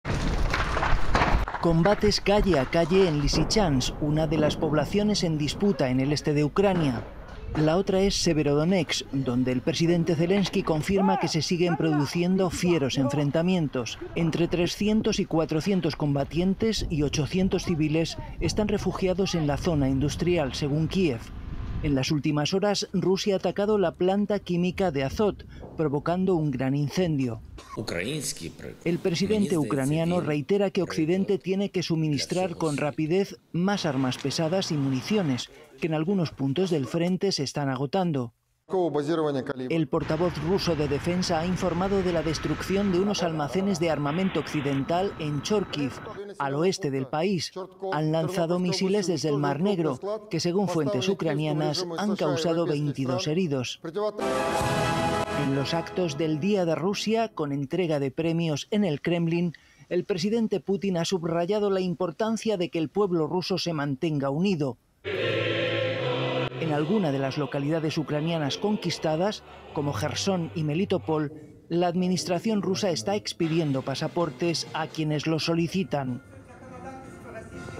GUERRA UCRANIA: RUSIA lanza un ATAQUE contra la PLANTA QUÍMICA de AZOT | RTVE Noticias
Los combates se centran en la localidad de Severodonetsk. Rusia ha lanzado un ataque contra la planta química de Azot, donde se refugian los pocos habitantes que quedan en la ciudad. El complejo industrial es el lugar más asediado de la localidad, como en su día lo fue la planta de Azovstal en Mariúpol, cuyas ruinas son el testimonio del bombardeo masivo ruso y de la encarnizada resistencia ucraniana. En Azot se refugian entre 300 y 400 civiles, que soportan en su interior los bombardeos rusos junto a las fuerzas ucranianas.